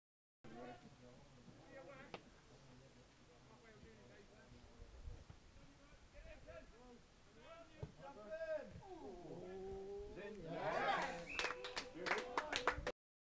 C.C,F.A Bond Timber Junior Cup 3RD Round Game, v St Stephen 10 v 0 Halsetown
match highlights Craig Coad shot is controlled by Courtney Rowe who scores St Stephen's first goal in the first minute of the game.